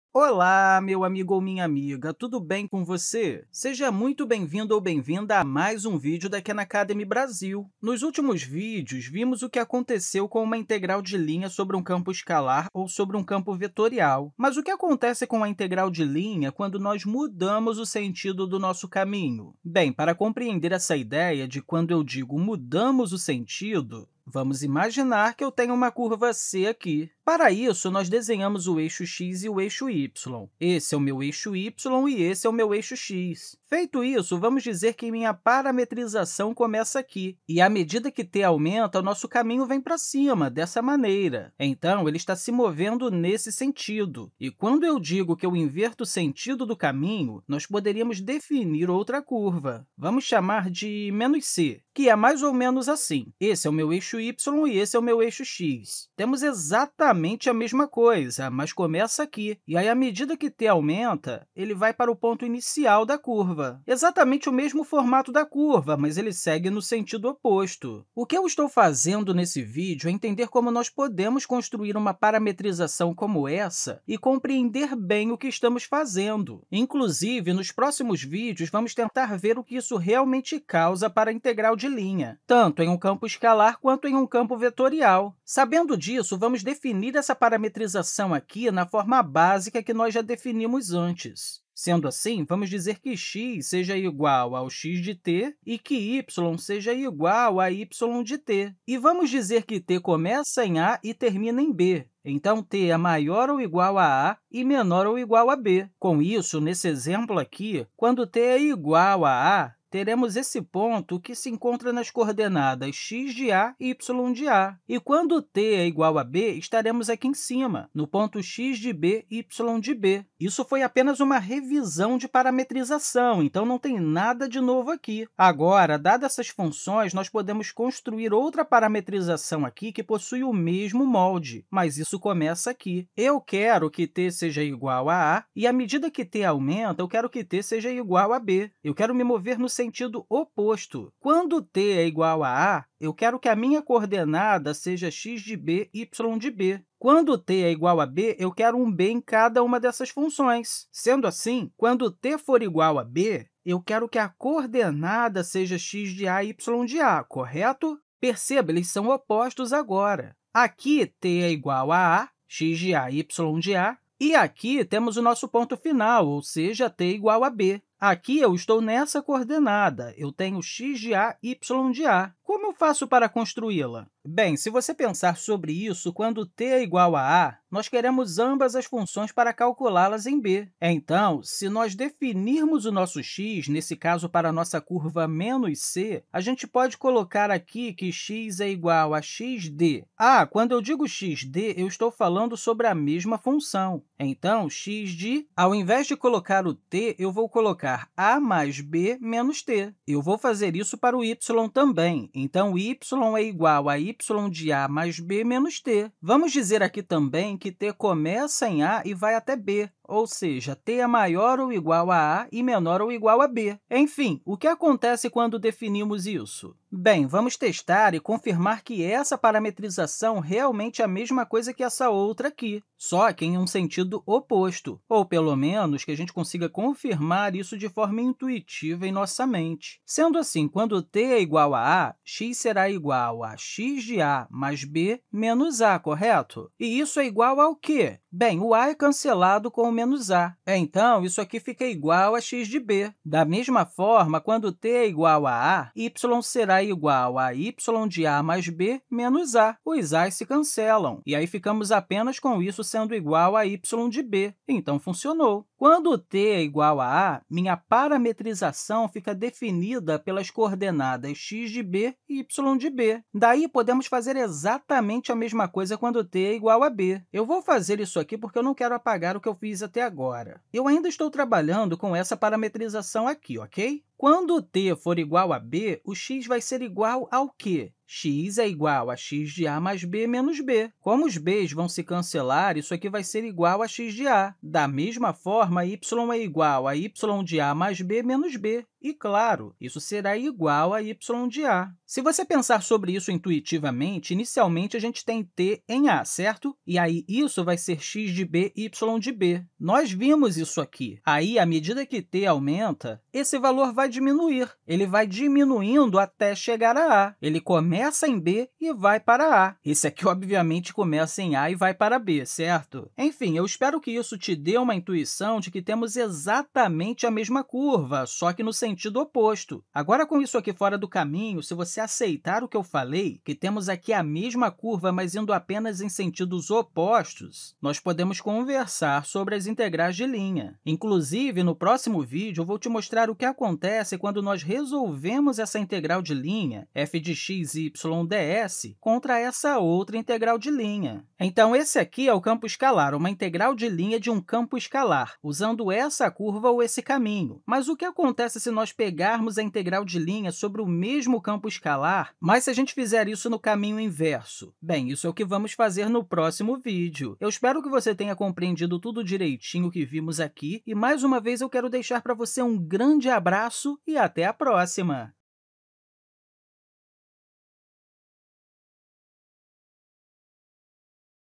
Parametrização de um caminho inverso
Entendendo como parametrizar um caminho reverso para a mesma curva.